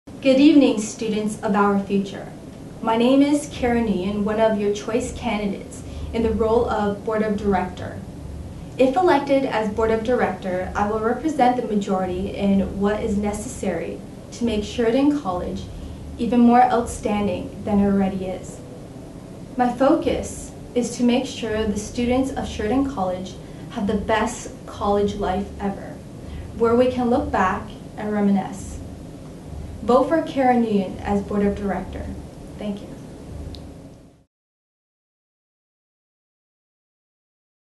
Karen Nguyen
Candidates for the 2011 Sheridan Student Union Elections from the Brampton Campus

Vote March 29, 30 and 31